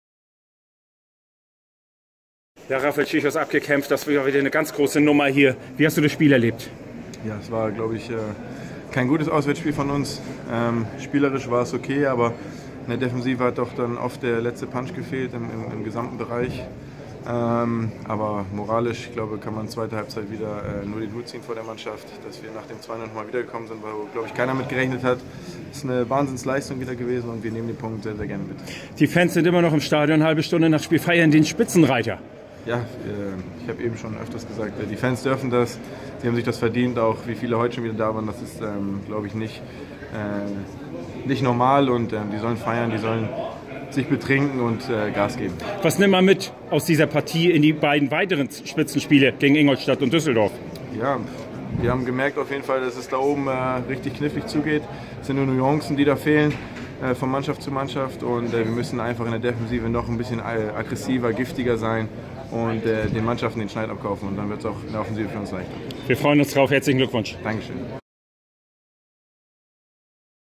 Rafael Czichos nach dem 2:2 beim 1. FC Nürnberg | KSVFCN | Holstein Kiel
Rafael Czichos im Interview mit Patrick Nawe.